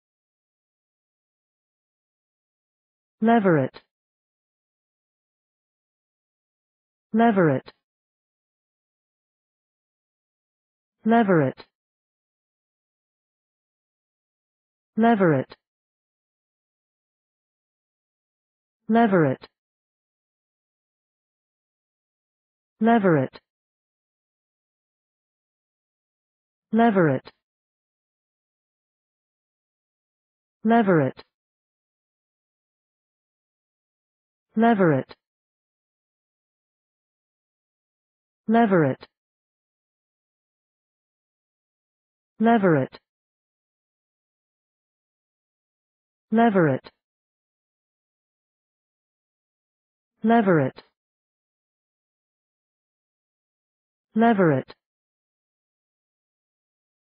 Pronunciation of Levirate | Definition of Levirate
Levirate pronunciation | How to pronounce Levirate in English?
/`levərɪt/
Meaning of Levirate | What is Levirate?
(1) (a.) Alt. of Leviratical
What is Levirate | Levirate meaning and definition?